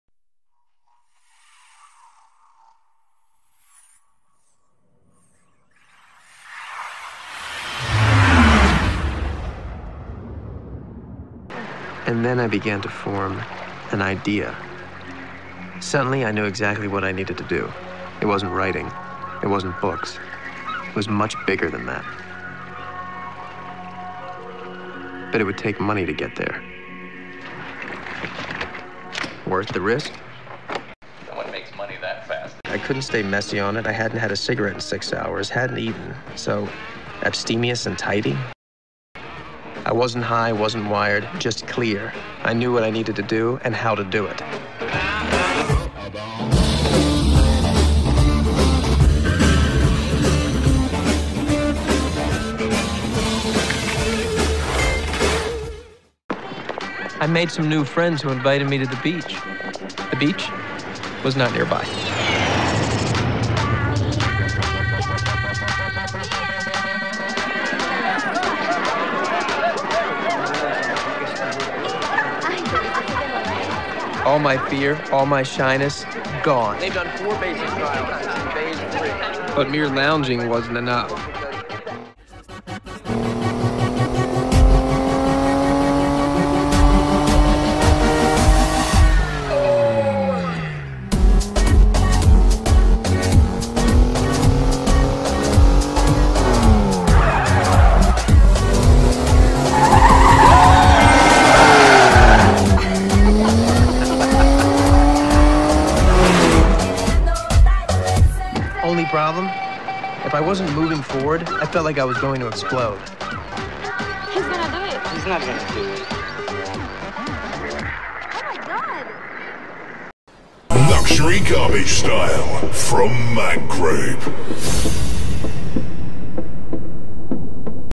Ecology lessons with Oleg Zhokhov. World: The Way of Samurai
Путь Самурая
Путь создателя Luxury Garbage Style
Путь Олега Жохова

Реальная история президента Mag group international

The Way of the Creator of LUXURY GARBAGE STYLE...
The Way of Oleg Zhokhov.

The true life story of the President of MAG GROUP International, Garbage Couturier - Oleg Zhokhov.

Проекты Олега Жохова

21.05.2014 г